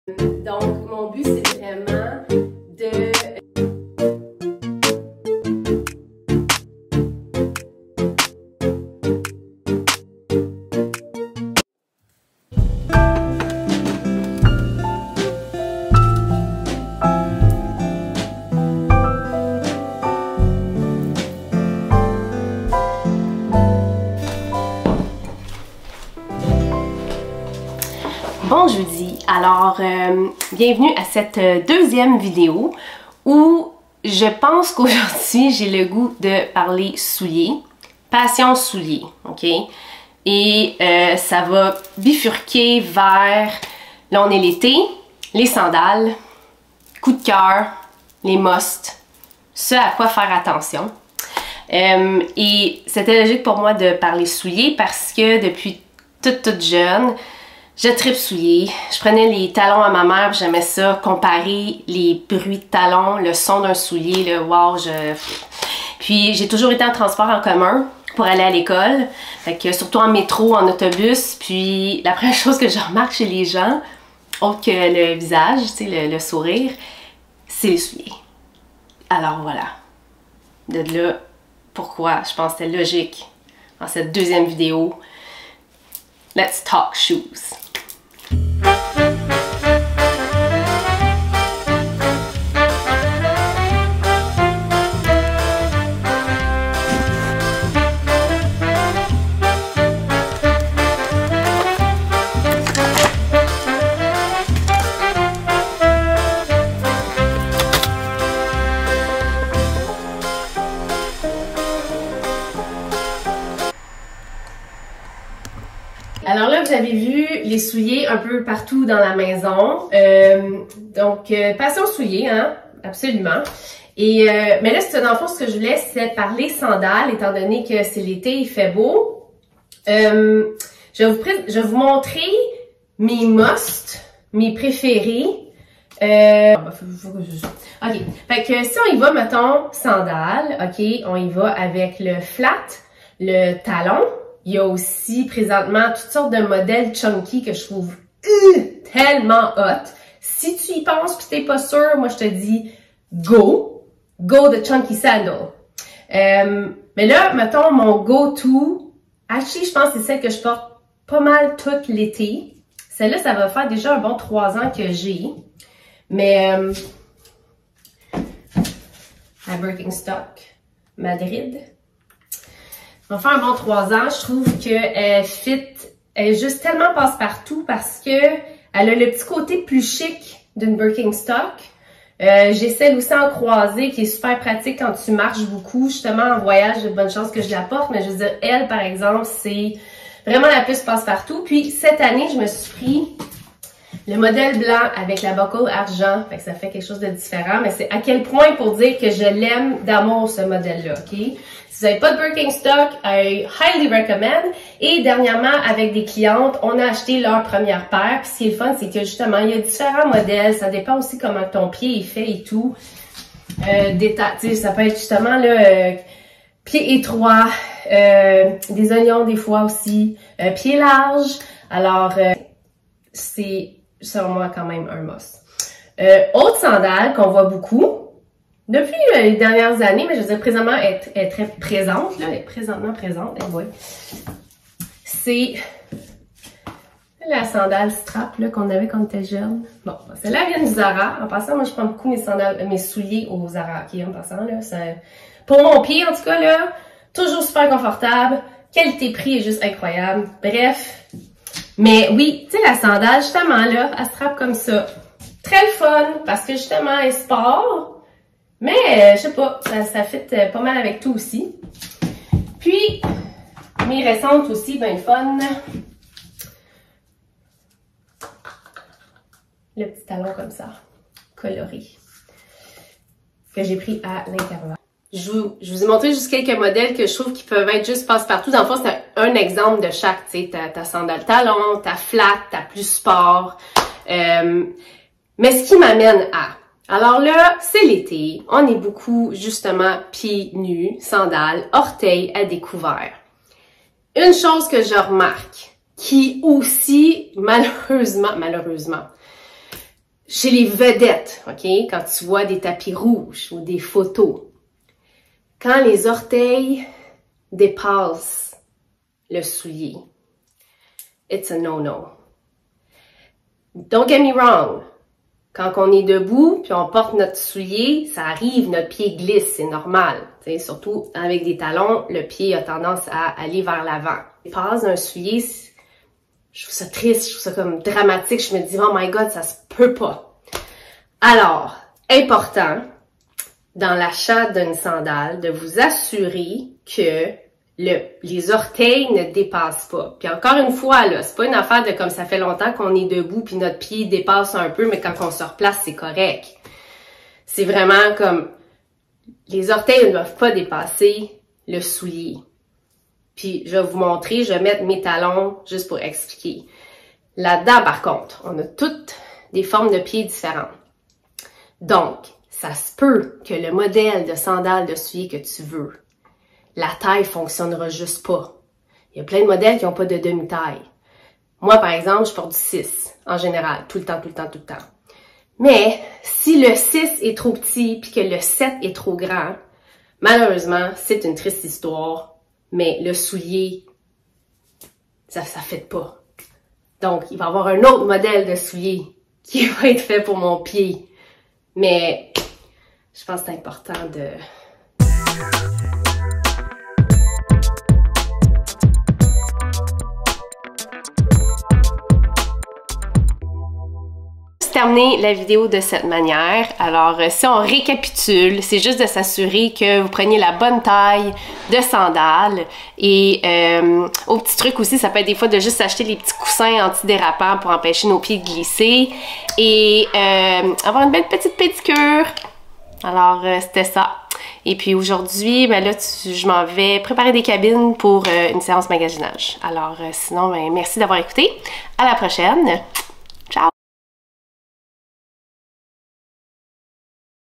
Vlog 2 - Parlons souliers
Salut! Dans cette capsule, nous jasons souliers, mais surtout sandales car c'est l'été. En sus, quelques conseils pour l'achat de votre prochaine paire. Bon visionnement et merci d'écouter :)